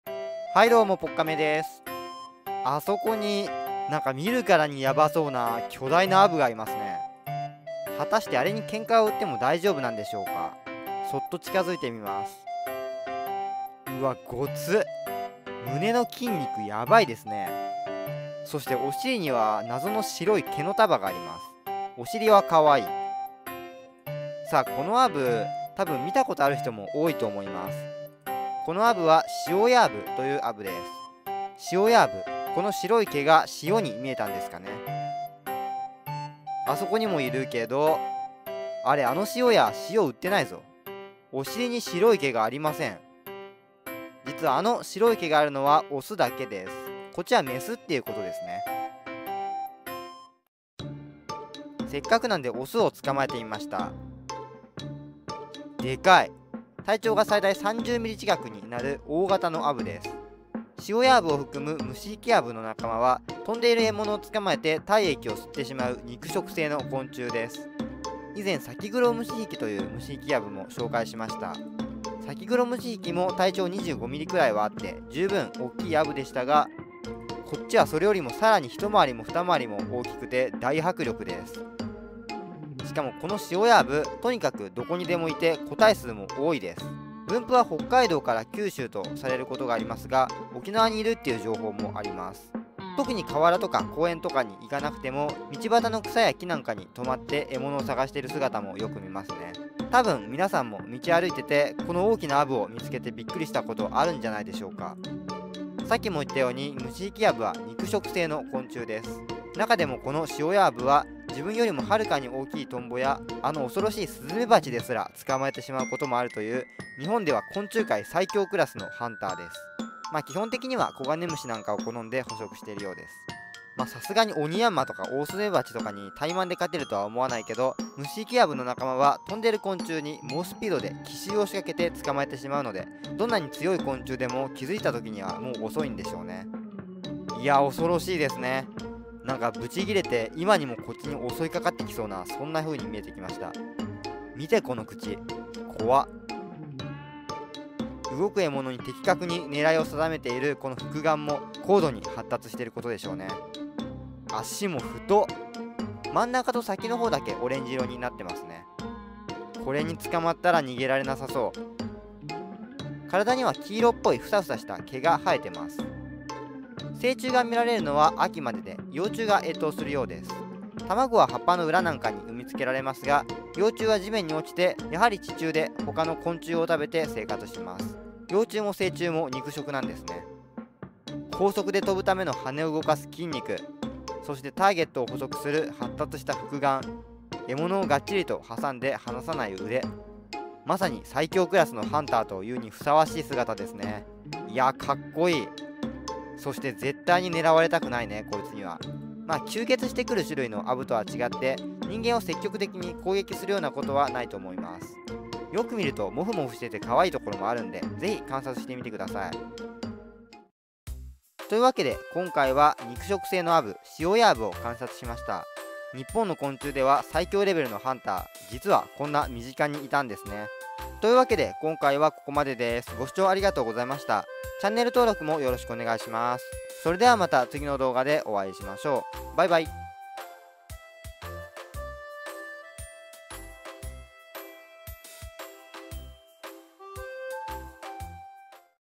スズメバチも標的に！？日本最強クラスの肉食アブ現る
今回の主役は…
シオヤアブ【Promachus yesonicus】
アオメアブとのライバル感は少年漫画